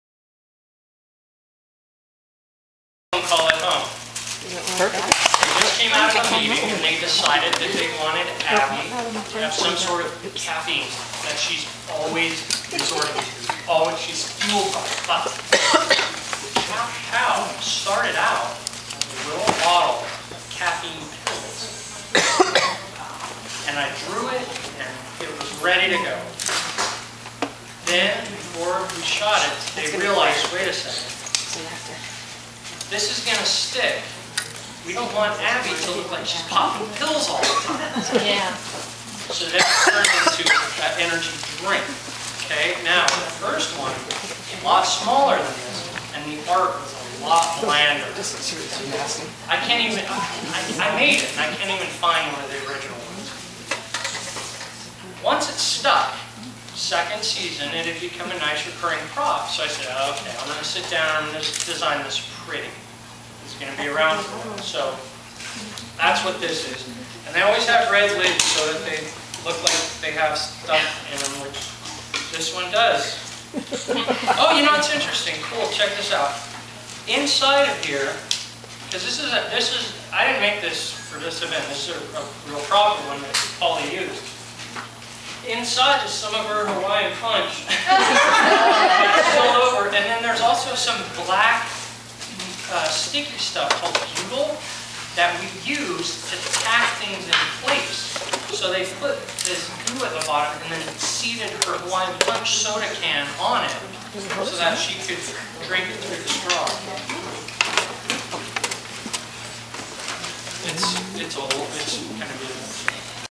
NCIS Fan Fest 2008 - Doug Reilly & Caf-Pow!
Clip of NCIS lead graphic artist Doug Reilly at the 3rd Annual NCIS Fan Fest. Here he is discussing the creation of the Caf-Pow